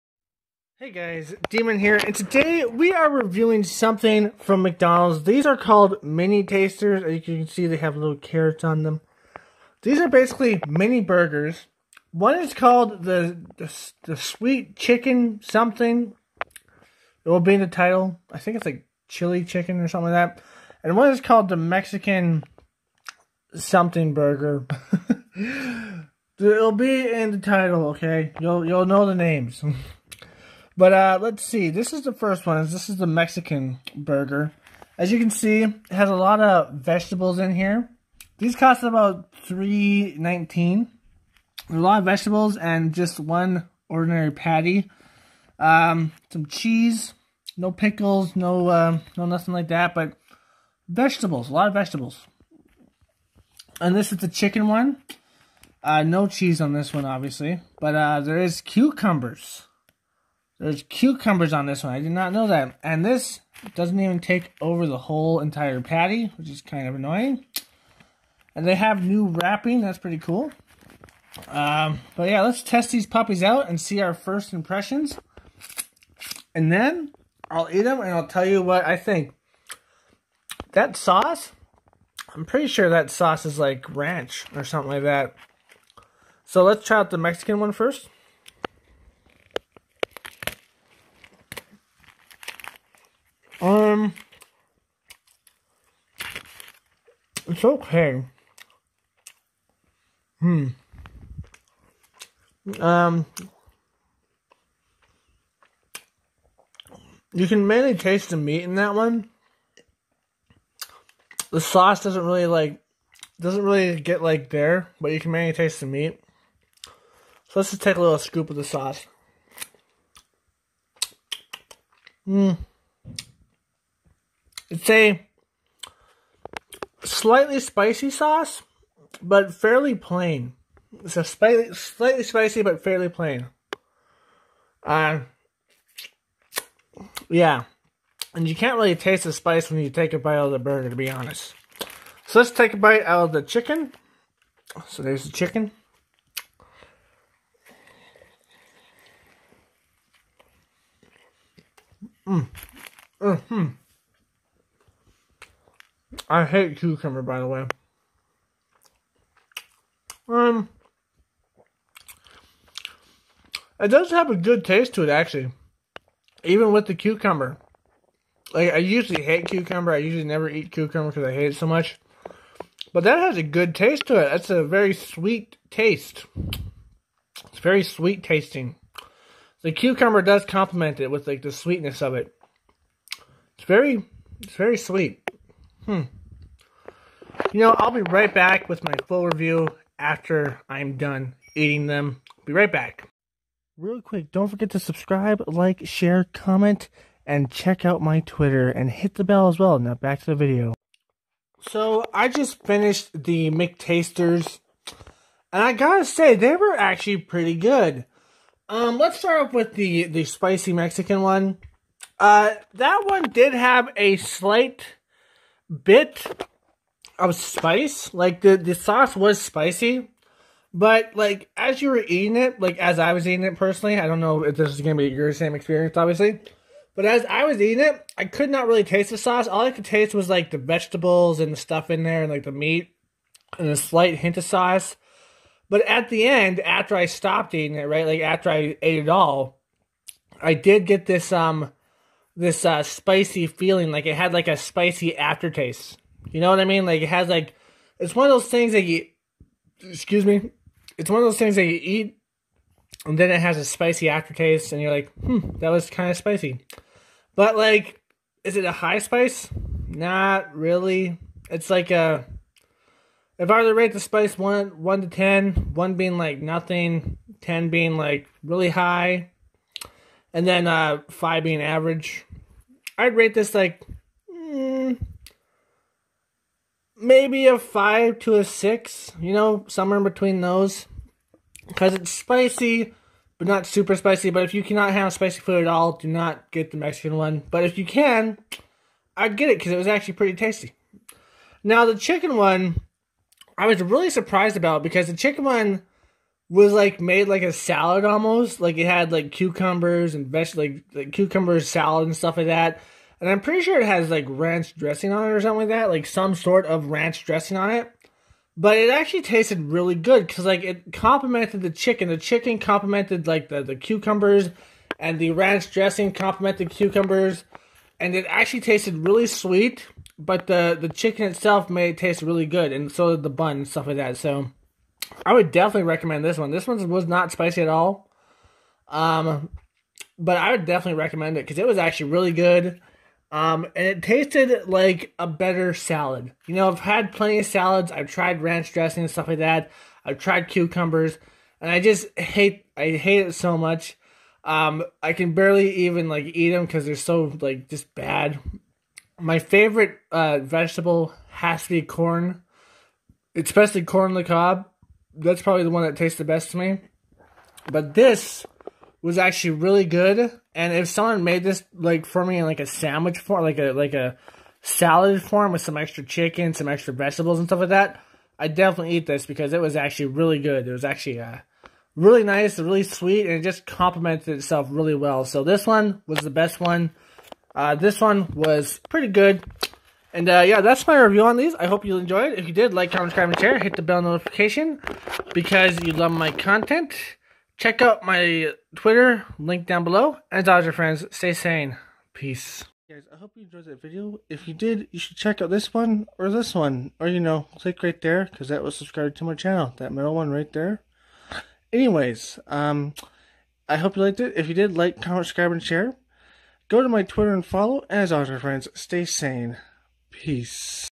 McDonald's McTasters Mexican Chipotle Burger & Thai Sweet Chili Chicken Sandwich Review
This is a food review a McDonald's review on their new Mexican Chipotle Burger & Thai Sweet Chili Chicken Sandwich My thoughts and opinions on these if they're good or bad delicious disgusting average or plain and more enjoy